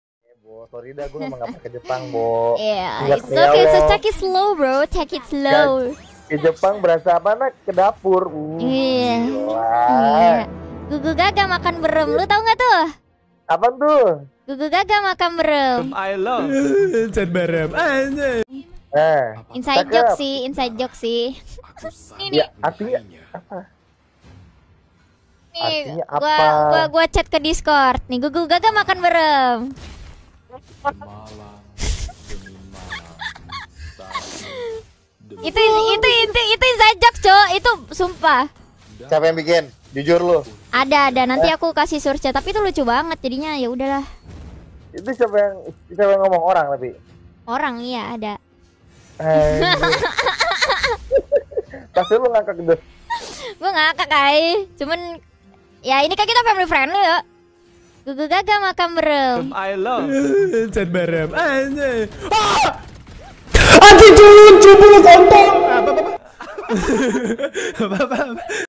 Kobo Ngasih Tau Jokes ACCLK Ke Evos Clover | [ Kobo Kanaeru & Evos Clover ]
Uwaduhhh Gugu Gaga Makan Berem

short
monetisasi